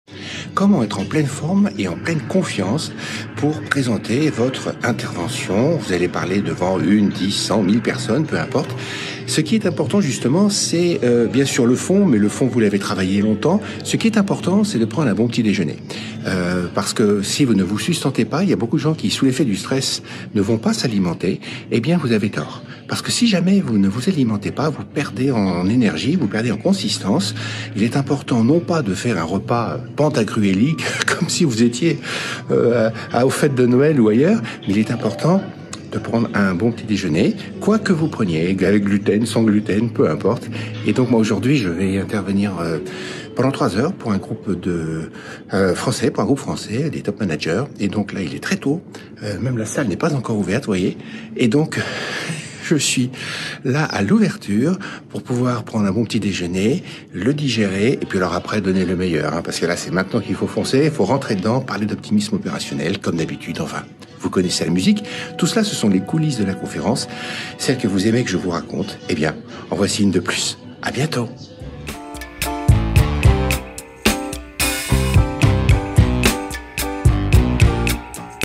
Avant de parler en public pensez a...
Comment être en pleine forme et en pleine confiance pour présenter votre intervention ?

Vous allez parler devant 1, 10, 100, 1000 personnes, peu importe. Ce qui est important justement, c'est bien sûr le fond, mais le fond, vous l’avez travaillé longtemps.

Ce qui est important, c'est...

▶ À propos :
- 6 ans d’études de management : France+ Japon + USA
👉 Et maintenant ?